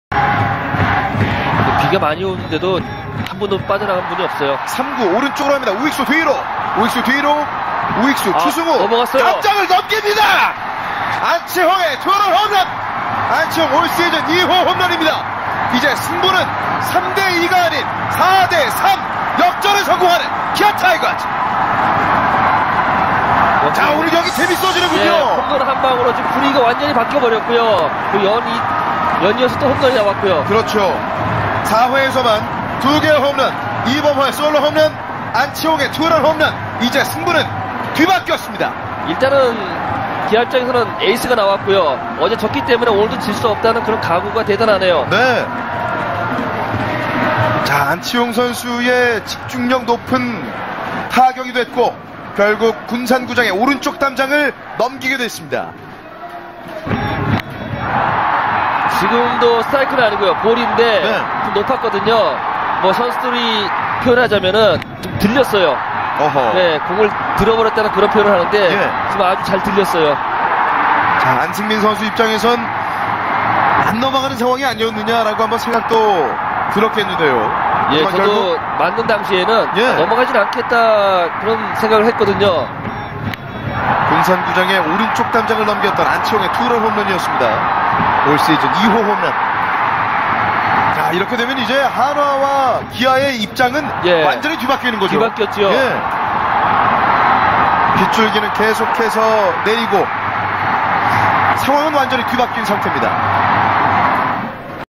20110521 KIA 안치홍 시즌 2호 홈런 (아프리카 버전)
KIA 기아 안치홍 시즌 2호 홈런 (아프리카 버전) @ 군산 한화전 (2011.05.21)